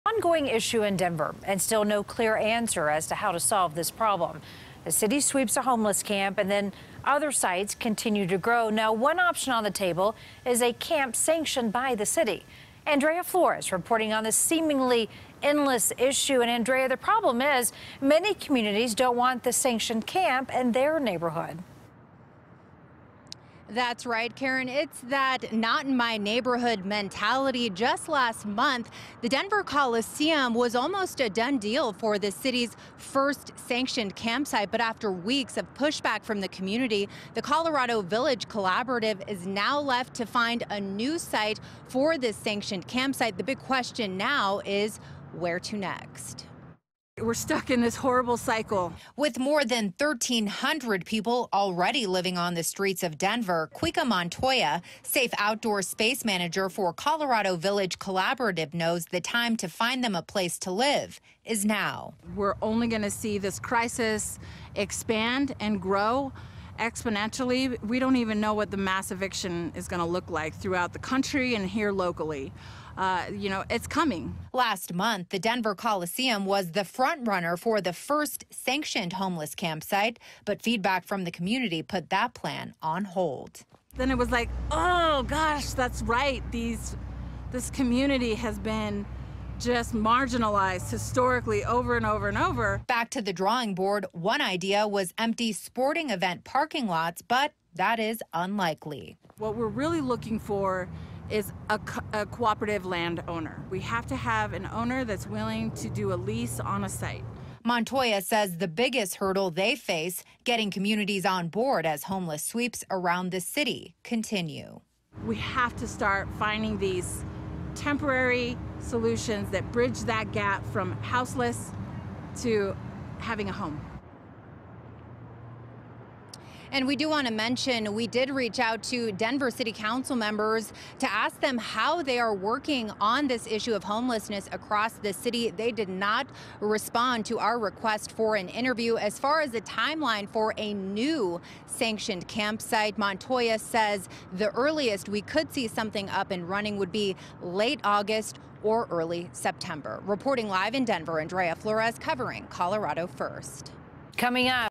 Denver Coliseum Out As Proposed Campsite For Those Experiencing Homelessness
The Colorado Village Collaborative is working to find a new place to house the homeless.